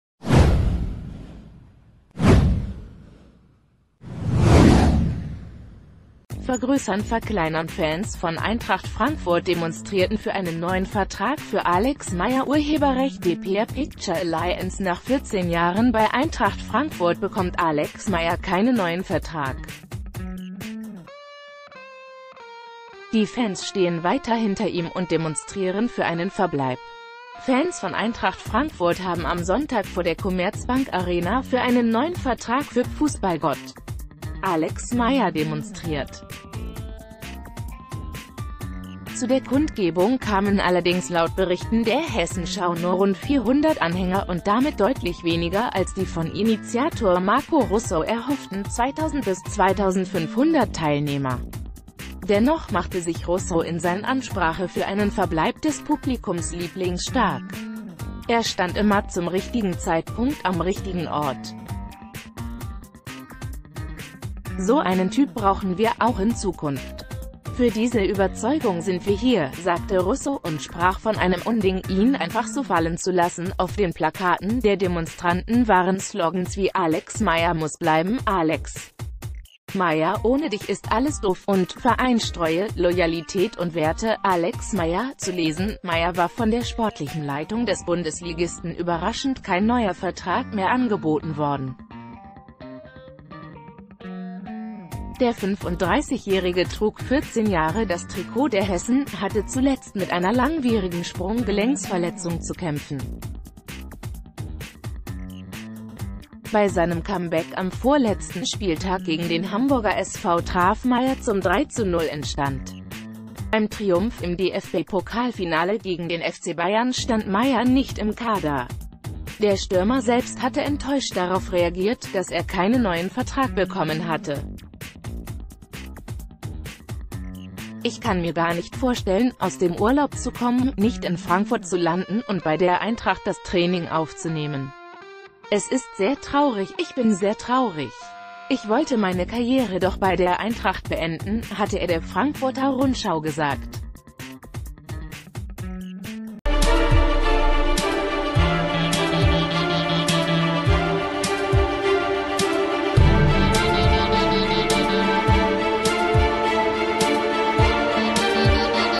Fans von Eintracht Frankfurt demonstrieren für Verbleib von Alex Meier
vergrößernverkleinern Fans von Eintracht Frankfurt demonstrierten für einen neuen Vertrag für Alex Meier © dpa Picture-Alliance Nach 14 Jahren bei Eintracht Frankfurt bekommt Alex Meier keinen neuen Vertrag. Die Fans stehen weiter hinter ihm und demonstrieren für einen Verbleib. Fans von Eintracht Frankfurt haben am Sonntag vor der Commerzbank-Arena für einen neuen Vertrag für "Fußballgott" Alex Meier demonstriert. Zu der Kundgebung kamen allerdings laut Berichten der hessenschau nur rund 400 An...